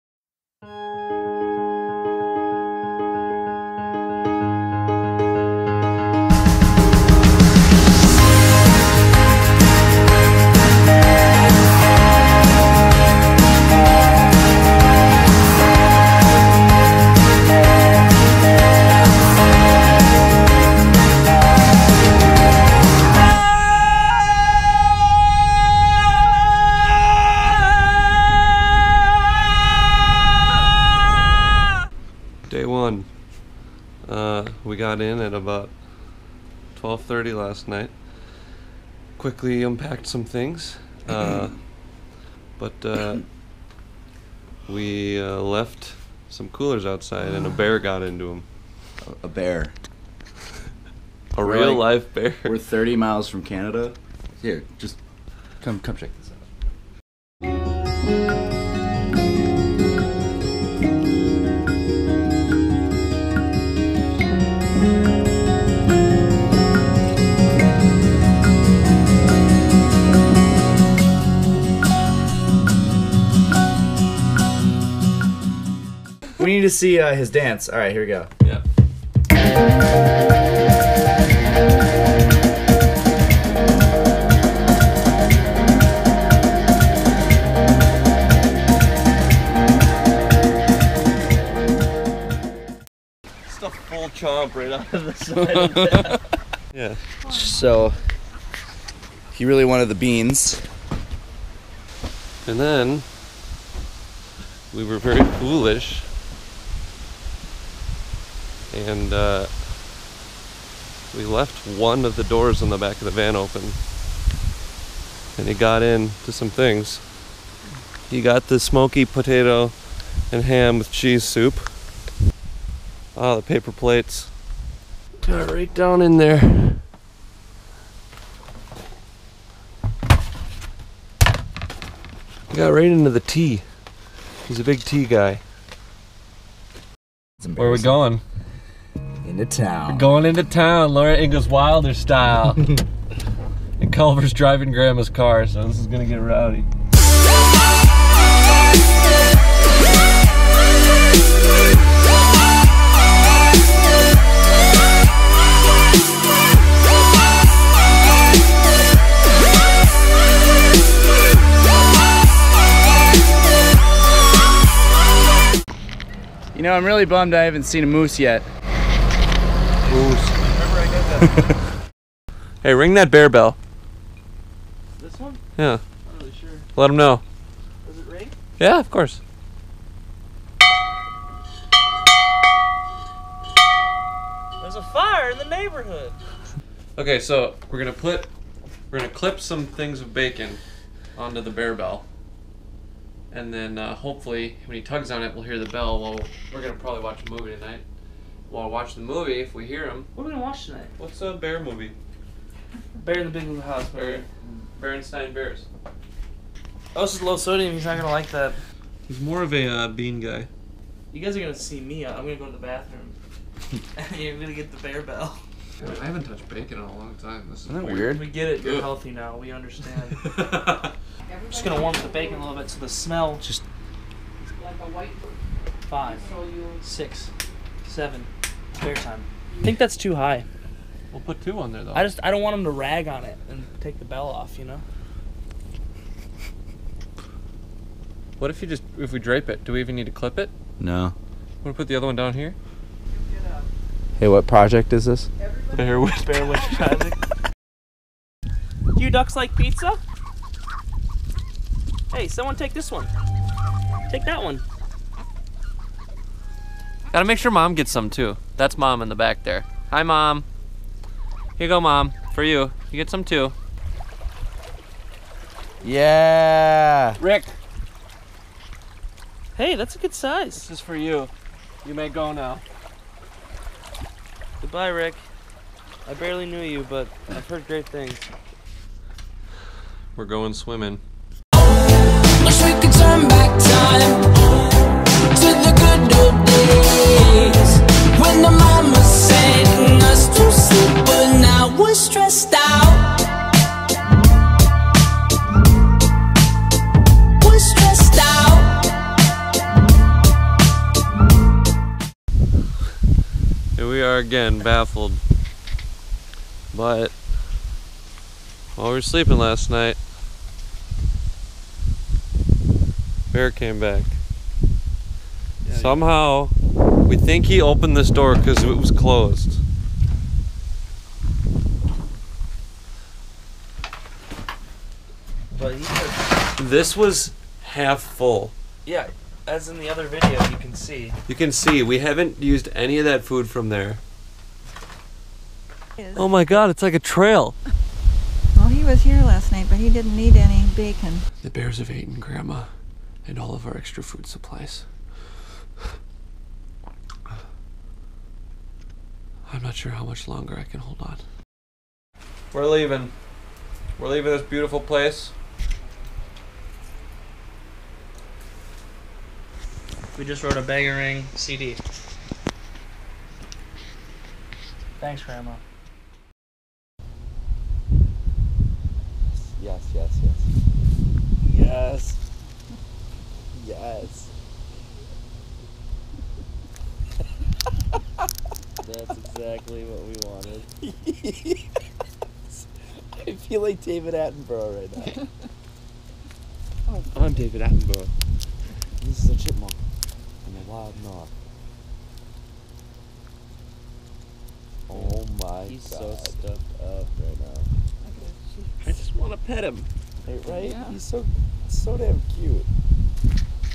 The Millenium | North Woods Chronicles
Venturing into the deep northern Minnesota woods to begin the process of making a new record, we took a much needed writing retreat at Feather's grandma's cabin to prepare for creating our next album. This is what happened...